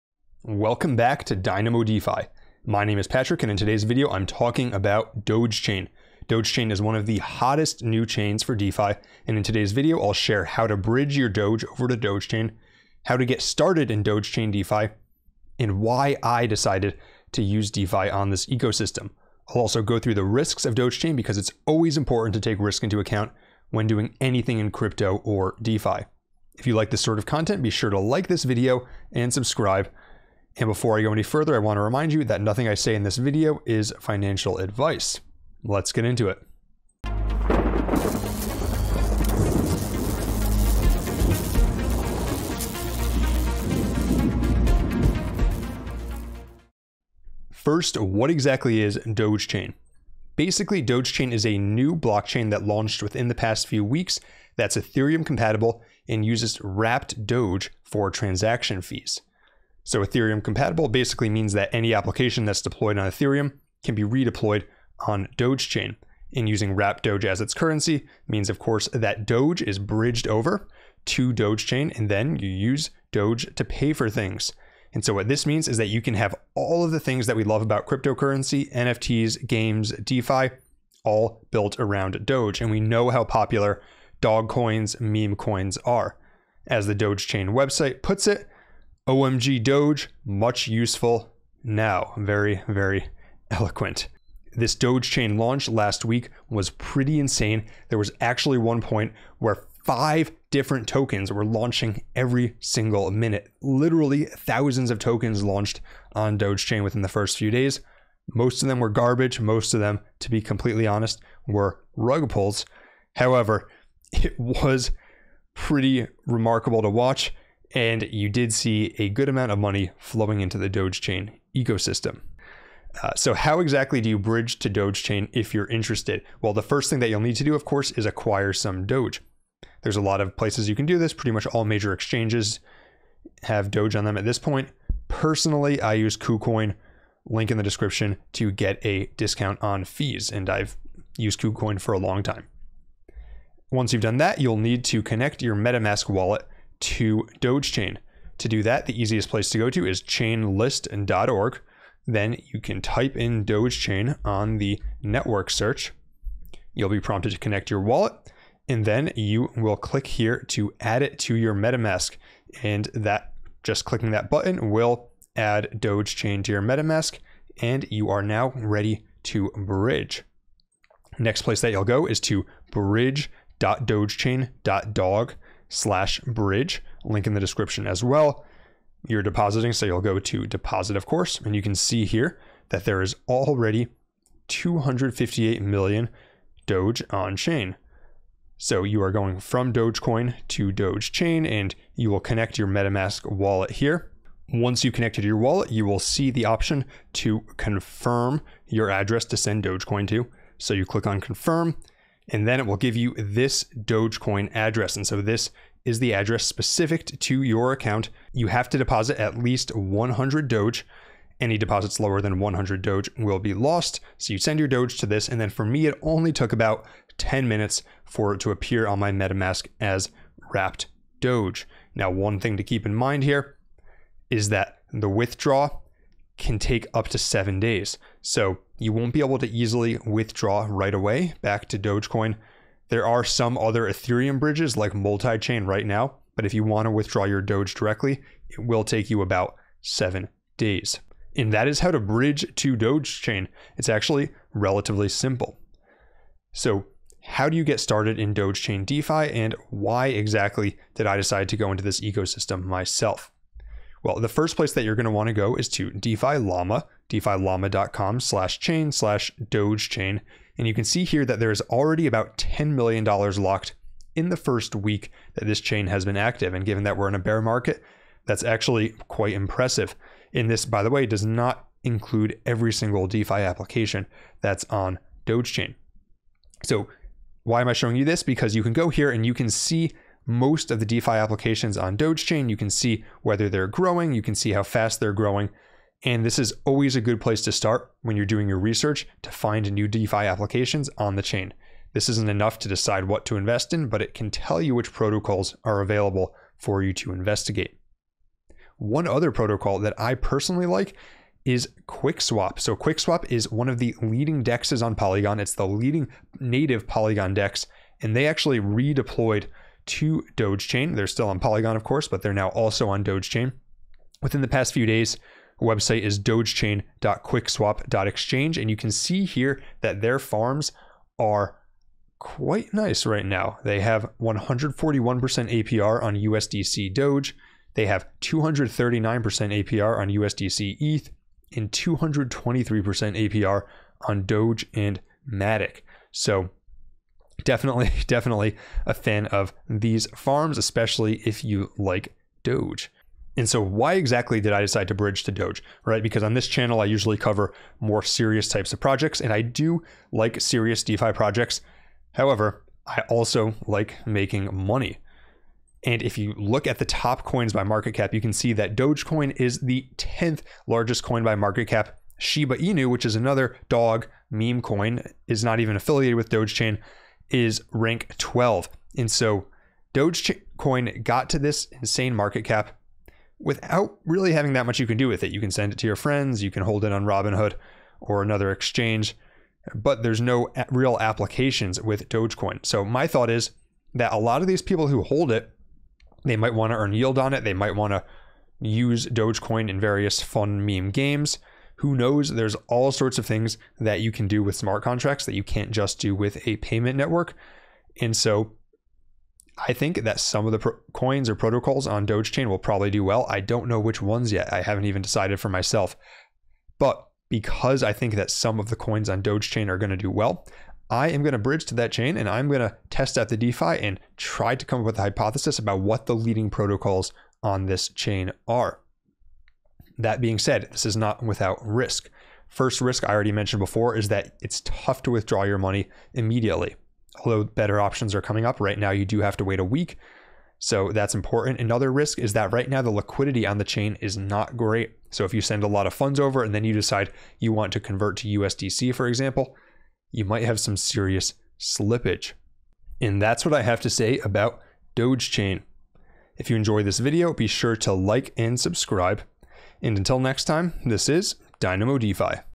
Dogechain: How to Bridge and Get Started
Dogechain a new chain that lets you build NFTs, DeFi, and Games on DOGE. In this video, I show you how to send your DOGE to Dogechain and how to get started with Dogechain DeFi.

0:00 Intro
0:37 Dynamo DeFi
0:47 What is Dogechain?
2:03 How to Bridge to Dogechain
4:06 Getting Started with Dogechain DeFi
5:43 Why I am Farming on Dogechain
7:00 Risks of Dogechain
7:23 Outro